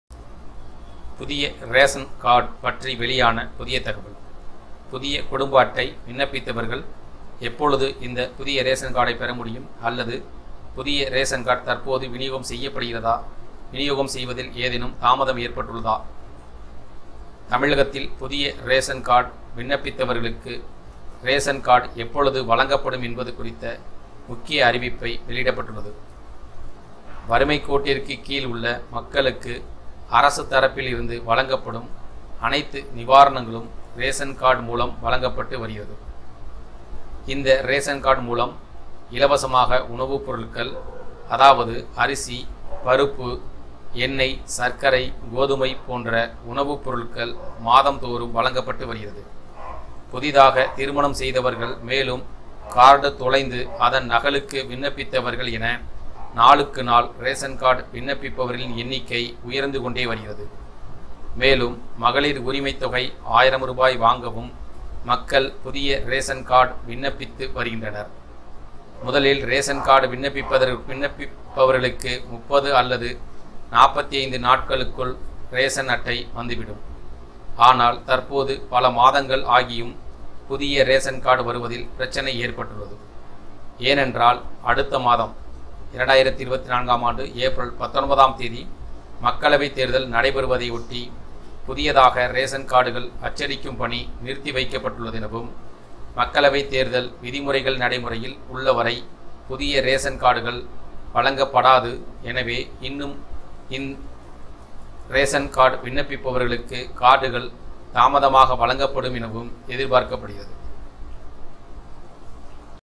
புதிய ரேஷன் கார்டு பற்றி வெளியான புதிய தகவல்
தற்போது பல மாதங்கள் ஆகியும் ரேஷன் கார்டு வருவதில் பிரச்சனை ஏற்பட்டுள்ளது. ஏனென்றால் அஅடுத்த மாதம் ஏப்ரல் 19ஆம் தேதி மக்களவை தேர்தல் நடைபெறுவதை ஒட்டி புதியதாக ரேஷன் கார்டுகள் அச்சடிக்கும் பணி நிறுத்தி வைக்கப்பட்டுள்ளது.